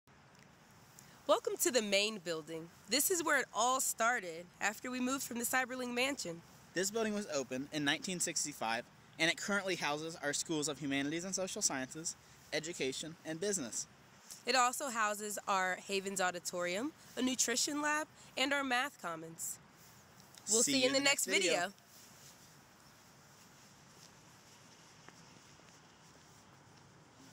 The Main Building
The oldest building on campus, the Main Building houses Havens Auditorium, a food lab, the Math Commons, and a majority of the Humanities and Social Sciences classes.